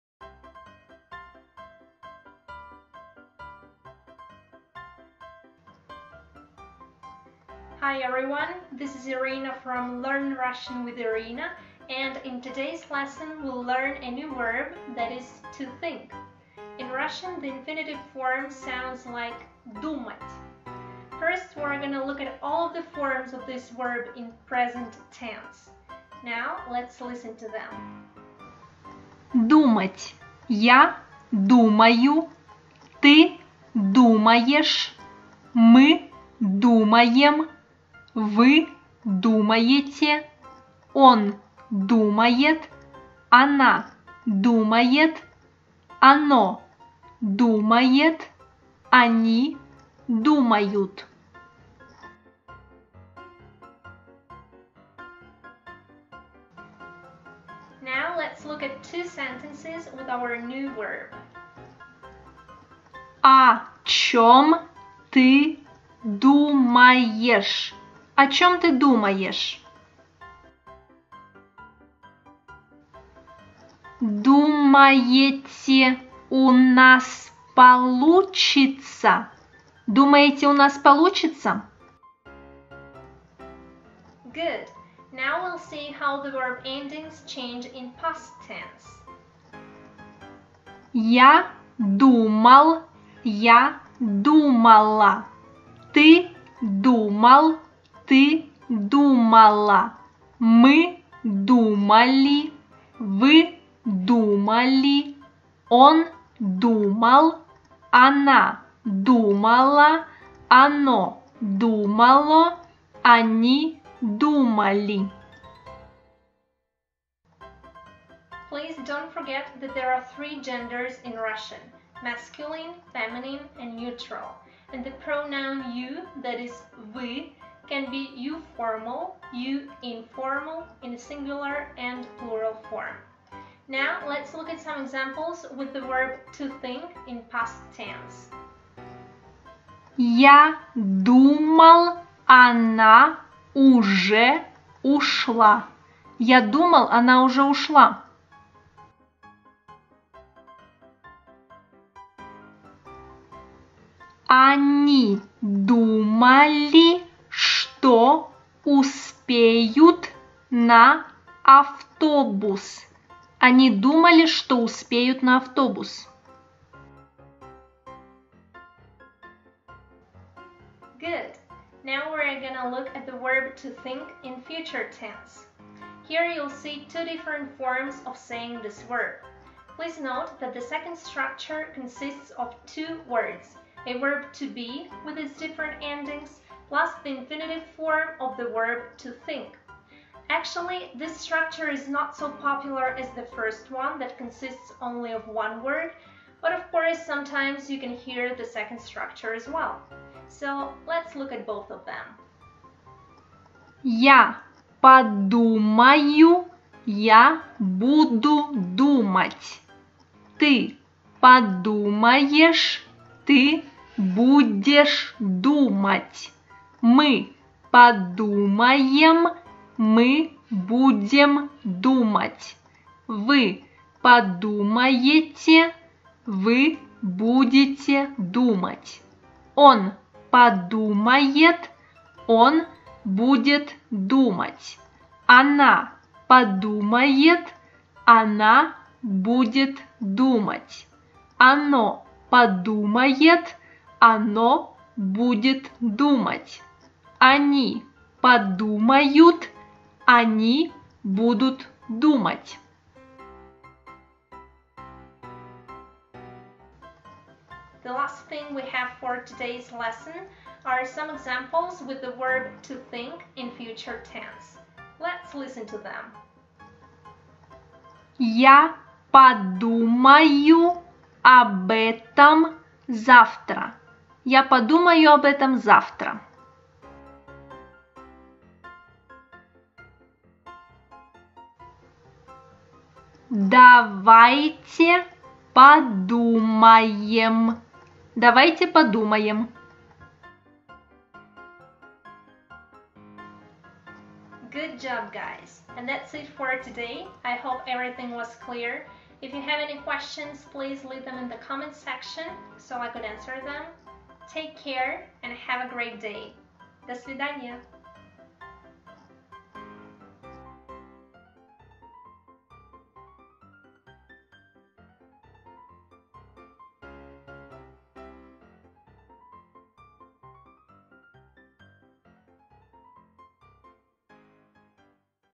Popular Russian Verbs Lesson #2 (tO THINK)
Present Tense
dumat = думать.

Ya dumayu. Я думаю.
Ty dumaesh. Ты думаешь.
My dumaem. Мы думаем.
Vy dumaete. Вы думаете.
On dumaet. Он думает.
Ona dumaet. Она думает.
Ono dumaet. Оно думает.
Oni dumayut. Они думают.

O chem ty dumaesh? О чём ты думаешь? What are you thinking about?
Dumaete u nas poluchitsya? Думаете у нас получится? Do you think we can handle it?

Past Tense

Ya dumal/dumala. Я думал/думала.
Ty dumal/dumala. Ты думал/думала.
My dumali. Мы думали.
Vy dumali. Вы думали.
On dumal. Он думал.
Ona dumala. Она думала.
Ono dumalo. Оно думало.
Oni dumali. Они думали.

Ya dumal ona uzhe ushla. Я думал она уже ушла. I thought she had gone already.
Oni dumali, chto uspeyut na avtobus. Они думали, что успеют на автобус. They thought they would have catch the bus.

Future Tense

Ya podumayu/budu dumat.
Ty podumaesh/budesh dumat.
My podumaem/budem dumat.
Vy podumaete/budete dumat.
On podumaet/budet dumat.
Ona podumaet/budet dumat.
Ono podumaet/budet dumat.
Oni podumayut/budut dumat.

Ya podumayu ob etom zavtra. Я подумаю об этом завтра. I'll think about it tomorrow.
Davayte podumaem. Давайте подумаем. Let's think.